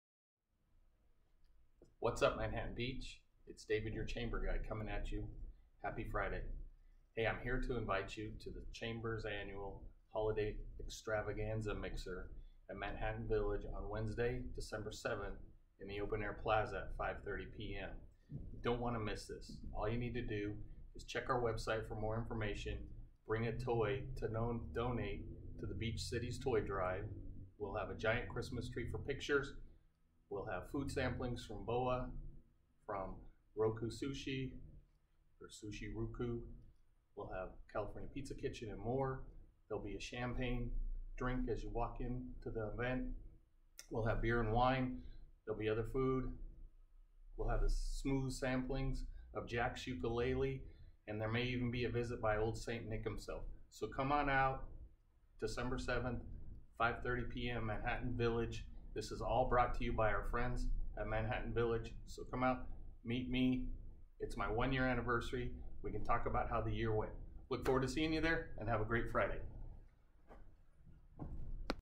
Manhattan Beach Holiday Mixer Extravaganza Invite 2022
Come join the Chamber in Celebrating the Holiday Season at our Mixer on Wednesday December 7, 2022, at Manhattan Village's Open Plaza at 5:30pm to 7:30pm